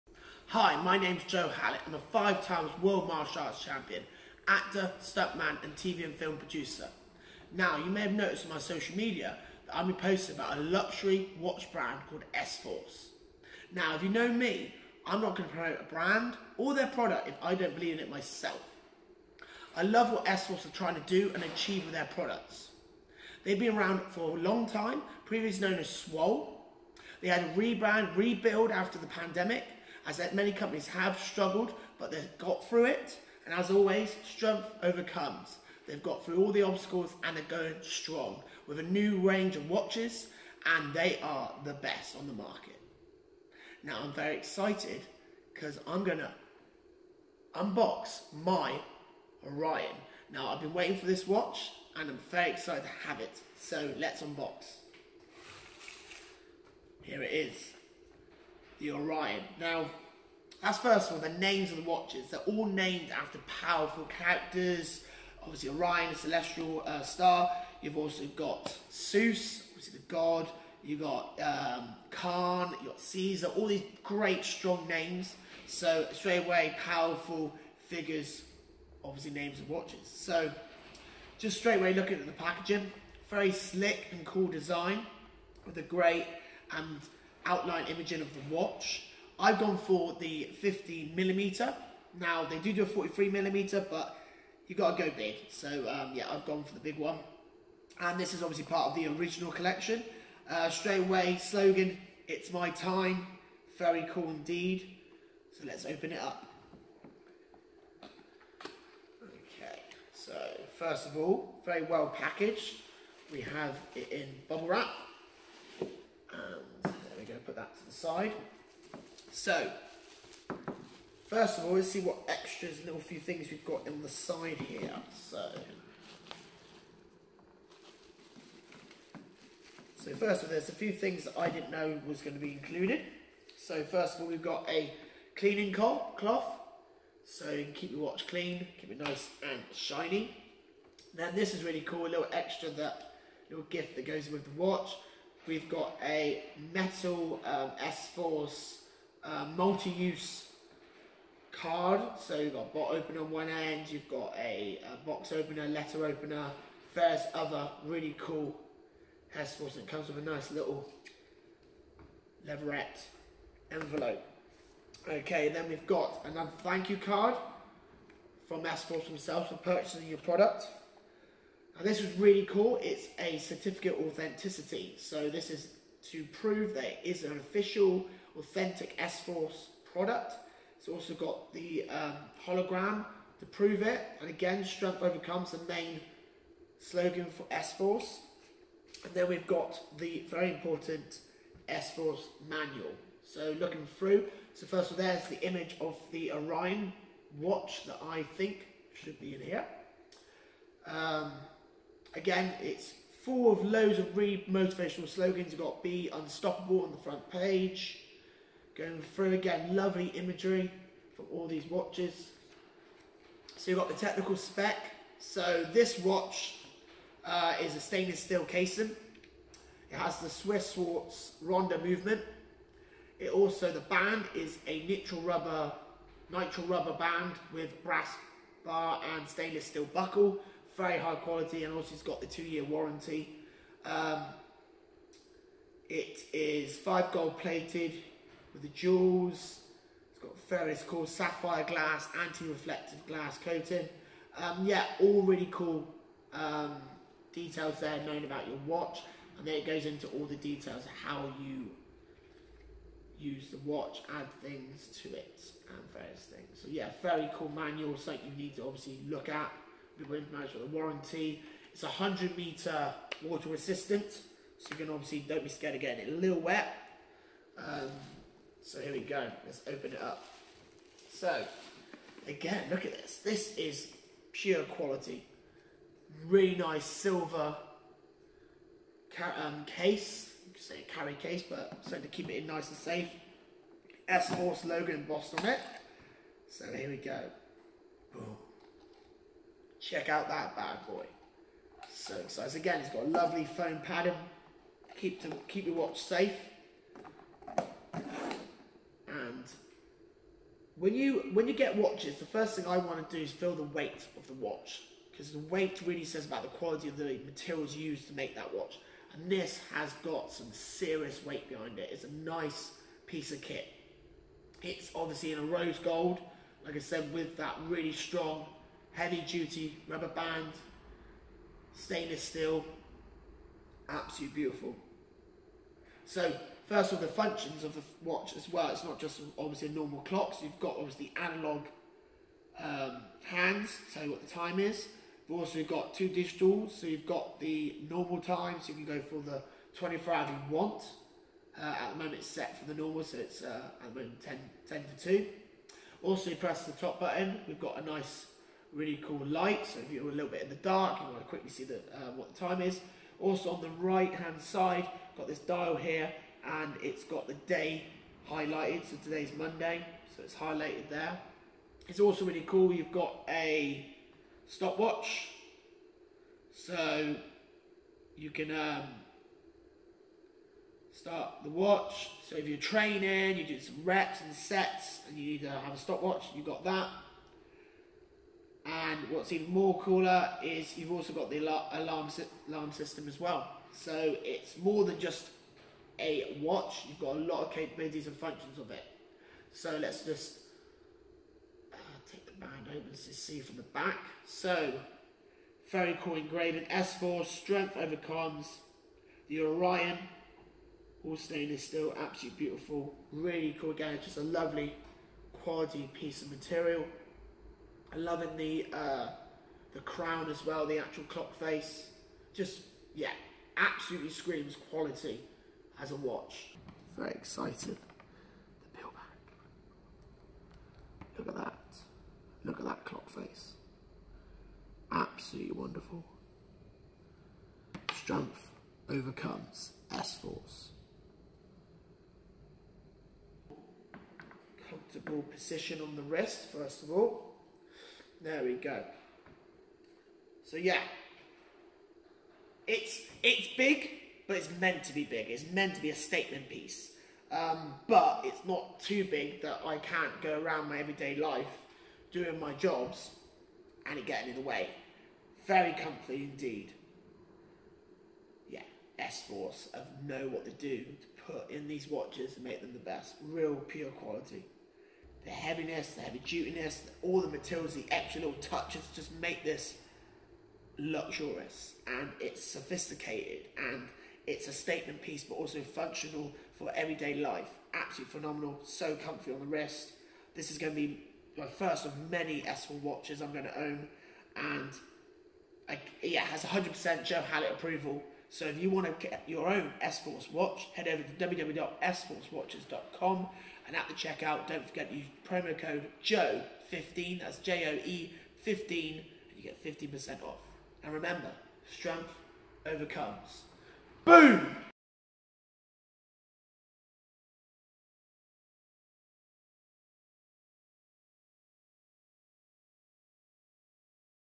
S-FORCE - ORION - Watch Unboxing
5X World Martial Arts Champion, Actor, Stuntman, TV/Film Producer and Director Joe Hallett takes the time to unbox his new luxury S-FORCE Watch the "Orion". Watch as he goes through the functions of the watch and explains why you got to own one of these luxurious heavy duty watches.

And don't forget "STRENGTH OVERCOMES"